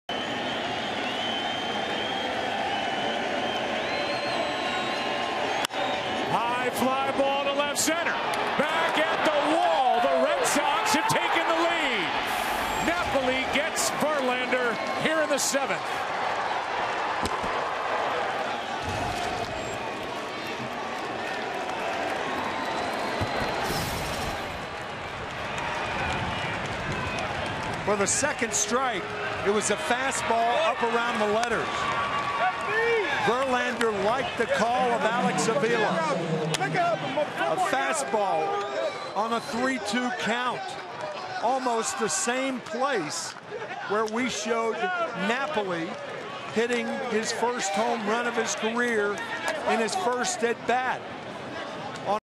2013/10/15 Napoli's solo homer
ALCS Gm3: Napoli breaks scoreless tie with homer